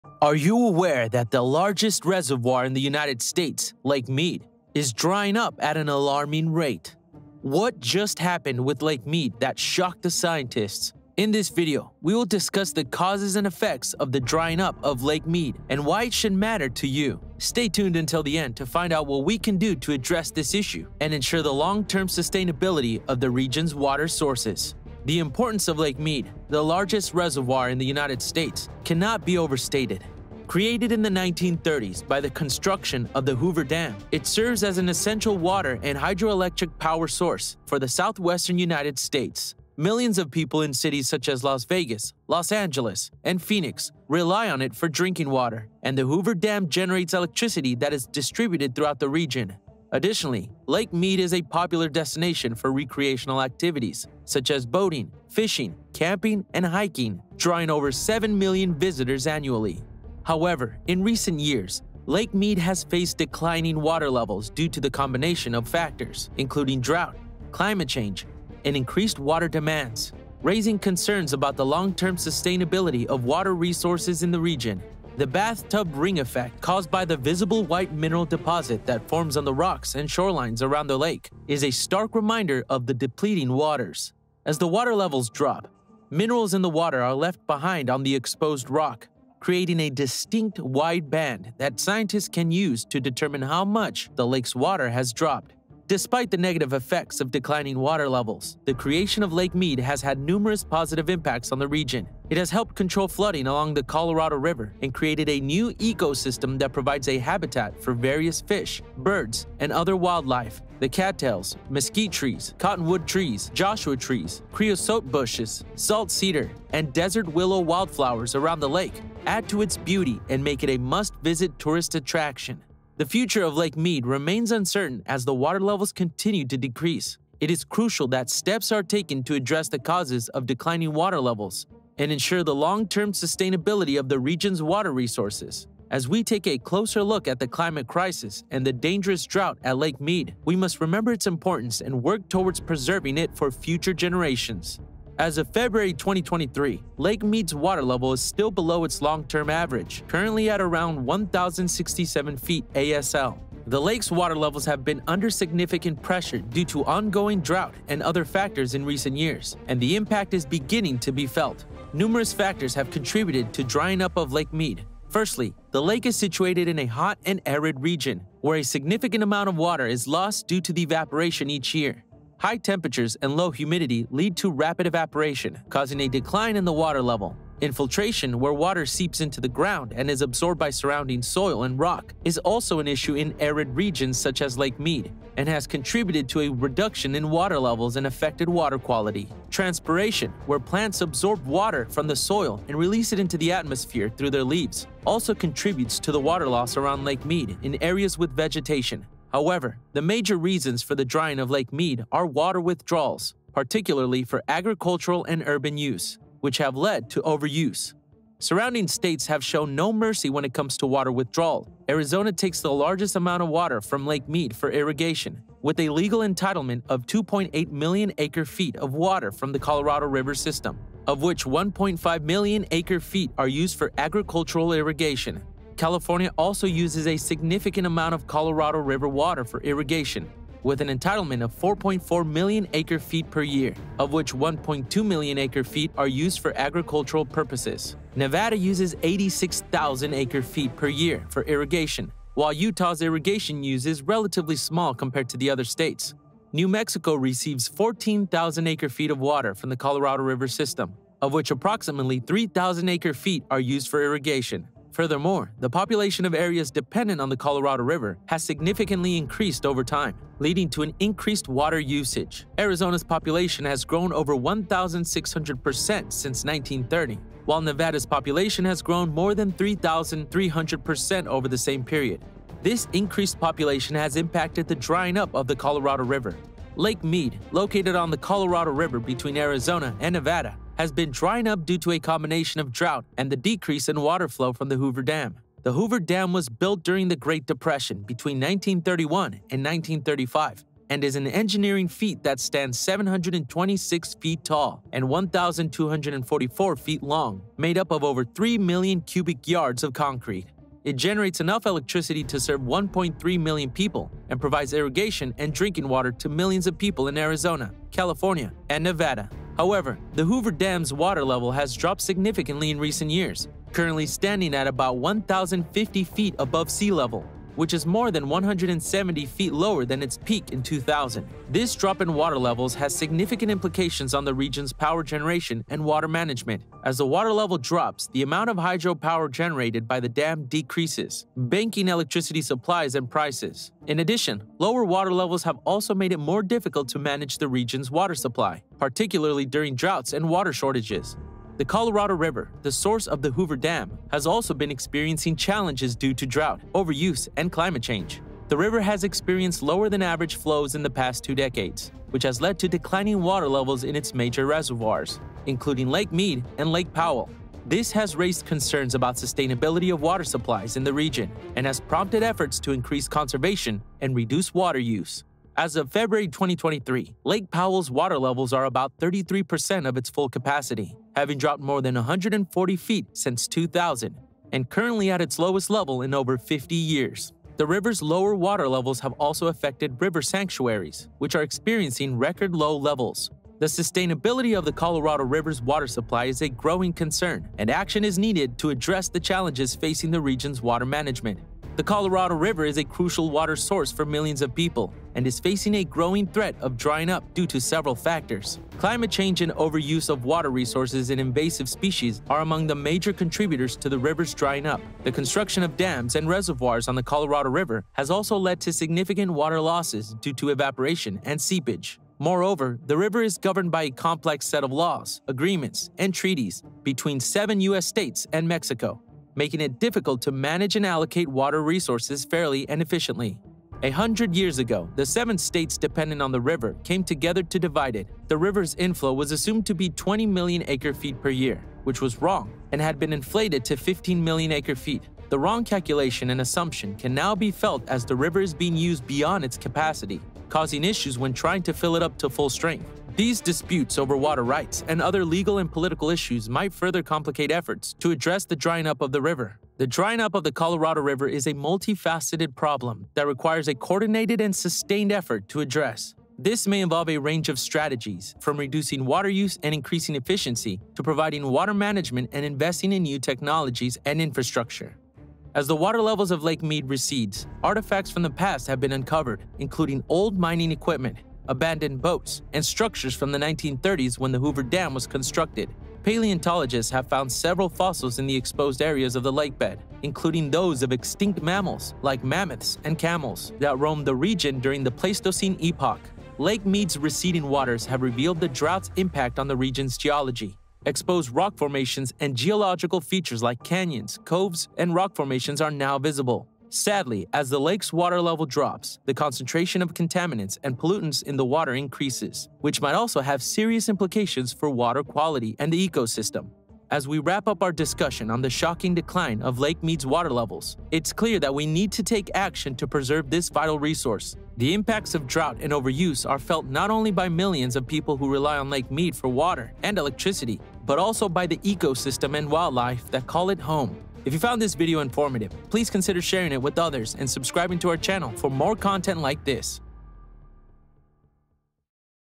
Lake Mead's Shocking Transformation: What's Happening to America's Iconic Reservoir?
Are you aware that the largest reservoir in the United States, Lake Mead, is drying up at an alarming rate? What JUST HAPPENED With Lake Mead that SHOCKED the Scientists? In this video, we will discuss the causes and effects of the drying up of Lake Mead and why it should matter to you.

The importance of Lake Mead, the largest reservoir in the United States, cannot be overstated. Created in the 1930s by the construction of the Hoover Dam, it serves as an essential water and hydroelectric power source for the southwestern United States. Millions of people in cities such as Las Vegas, Los Angeles, and Phoenix rely on it for drinking water, and the Hoover Dam generates electricity that is distributed throughout the region. Additionally, Lake Mead is a popular destination for recreational activities such as boating, fishing, camping, and hiking, drawing over 7 million visitors annually.